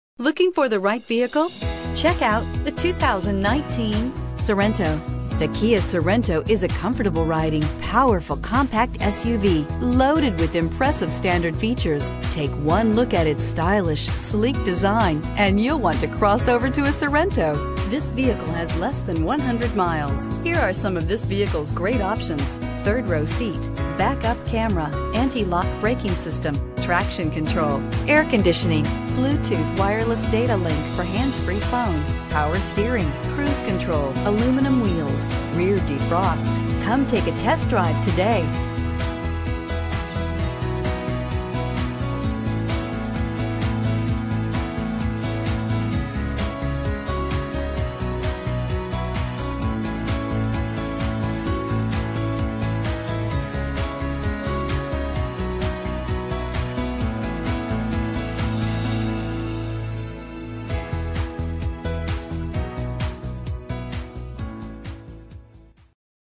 2019 Kia Sorento Monroe NC 97197
2019 Kia Sorento L

Pick up this Sparkling Silver 2019 Kia Sorento, available today at Union County Kia. This could be the one you've been searching for.